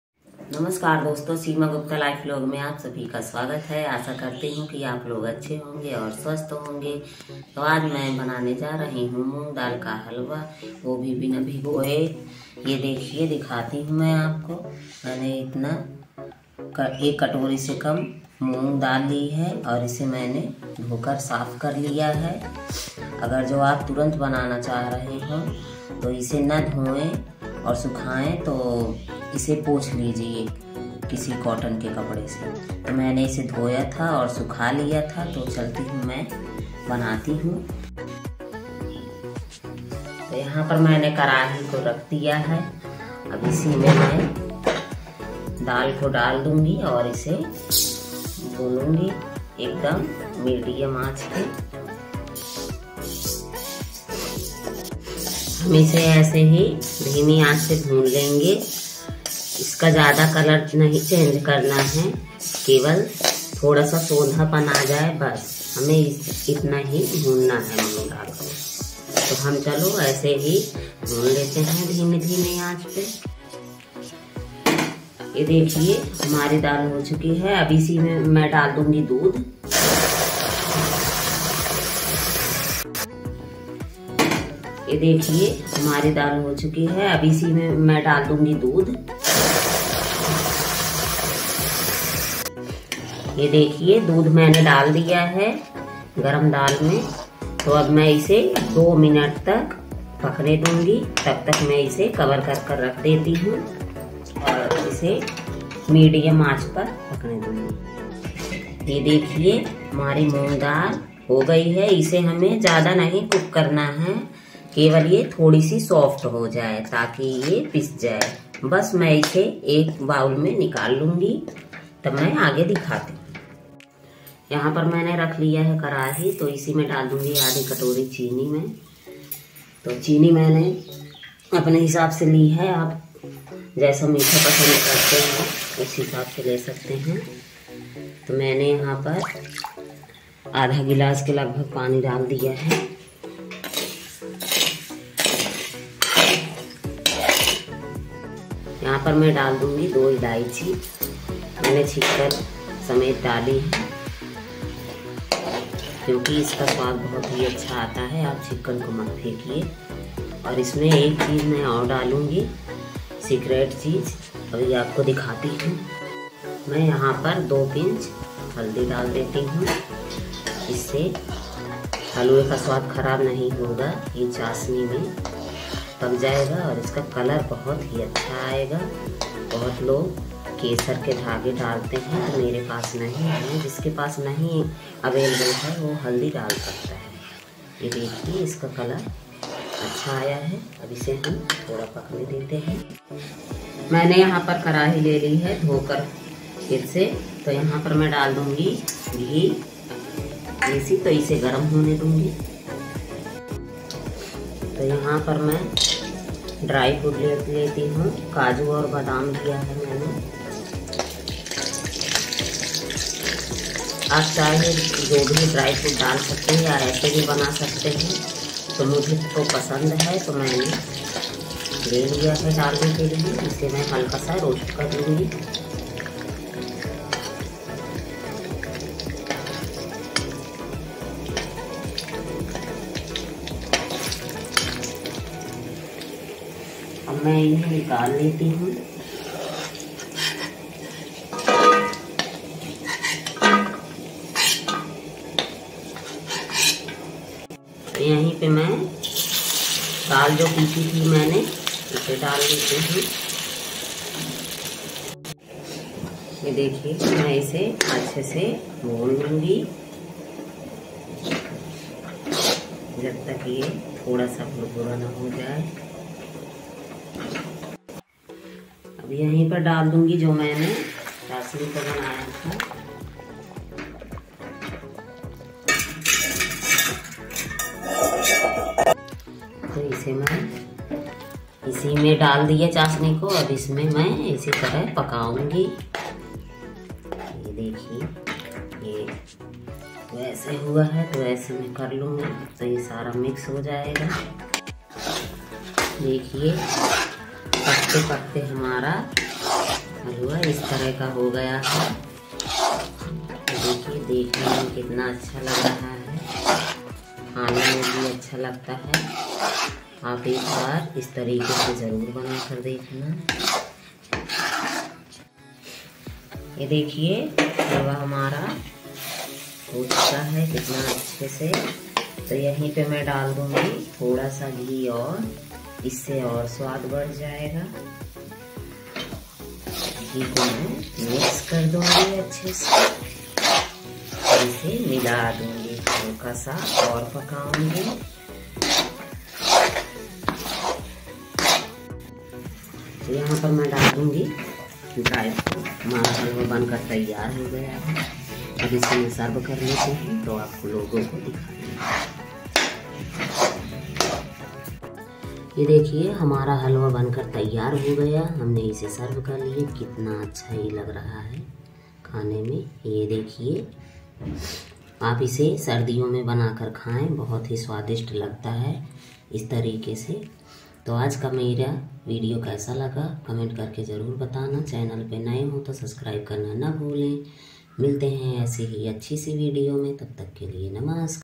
मूंग दाल हलवा बनाने का बिल्कुल नया आसान तरीका आजसे पहले आपने कभी नही देखा होगा
how to make moong dal halwa in instant pot